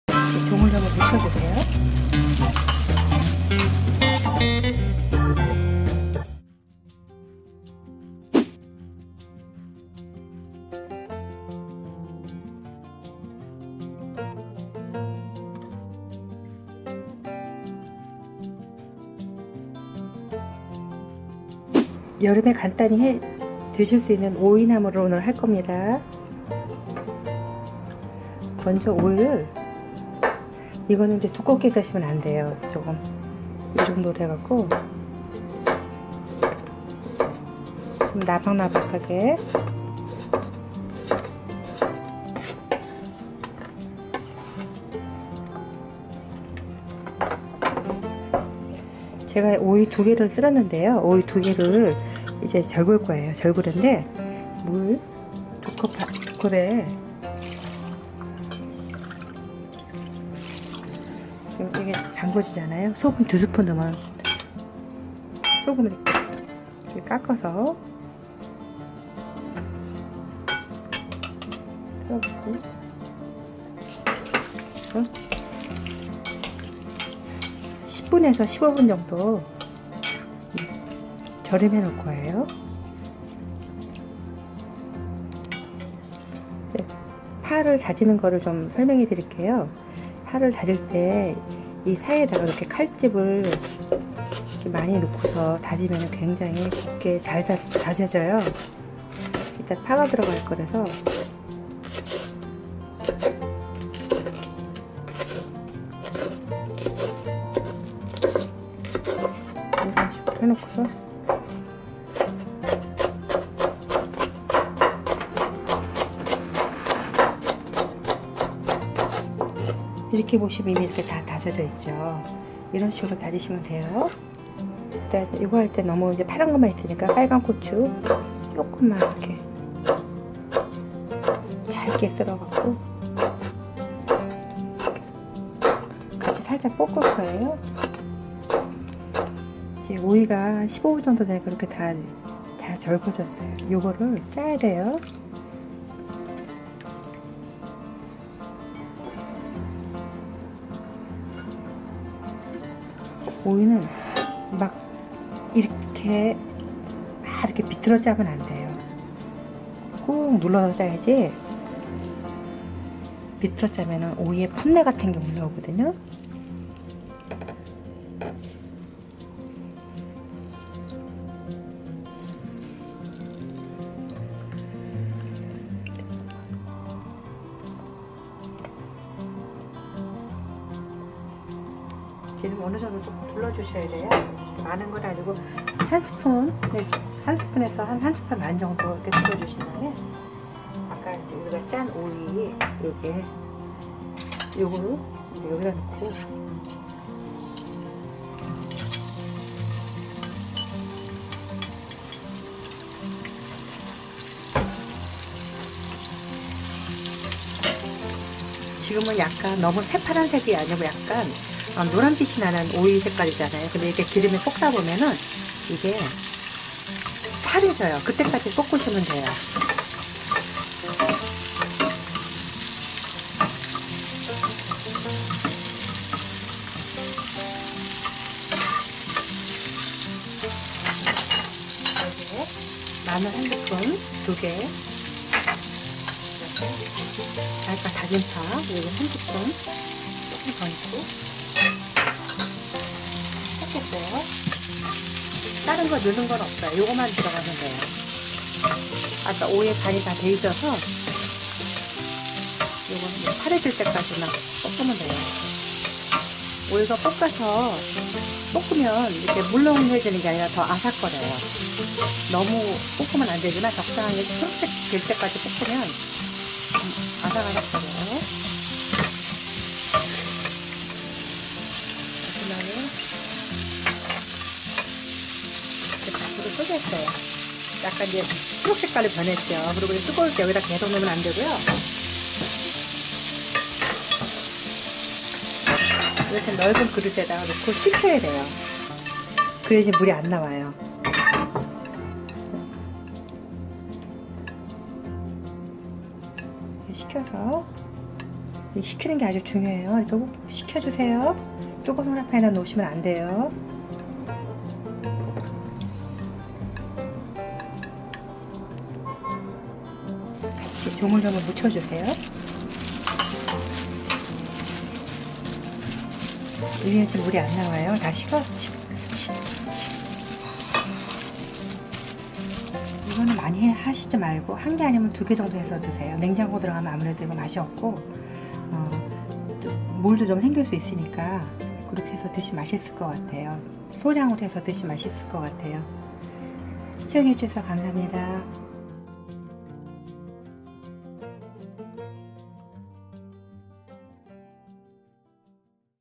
아삭아삭한 오이무침 오이나물 맛있게 만드는 방법 | 오이무침 오이나물 황금레시피
'딸을 위한 레시피' 밴드에서 더 가깝게 만나요^^

✅영상에서 언급한 컵, 스푼은 계량도구입니다.
   큰술:Tablespoon(15ml)
   작은술:teaspoon(5ml)
   1컵(Cup)= 200ml (200g이 아니예요)
✅영상의 상업적 이용, 2차 편집 및 재 업로드를 금지합니다

✅재료
오이2개, 물2컵, 소금2T, 참기름1T, 마늘1T, 다진대파1T,
깨소금1T, 홍고추약간

⚠️이 영상은 ‘딸을위한레시피’에서 제작하였으며,
딸을위한레시피의 저작물을 동의없이 무단으로 침해할 경우